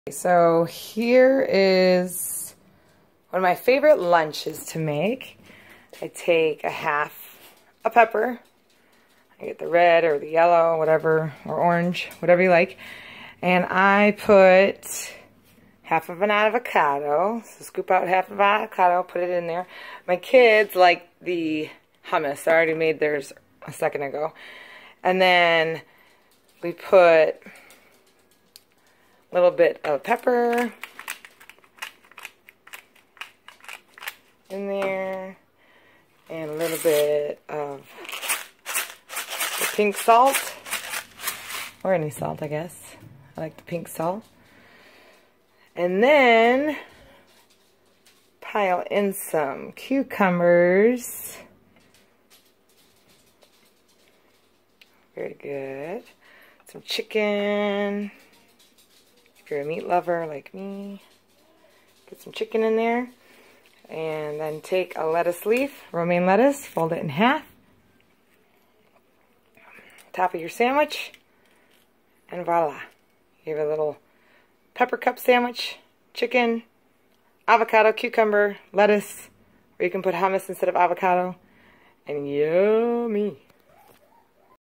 5min. Healthy Lunch Dinner Idea Under Pepper cup Sandwich 200 calories
This is an easy 5 minute lunch or dinner idea if you already have chicken cooked or you can substitute with any other protein or keep vegetarian removing the meat.  Under 200 calories and fills you up!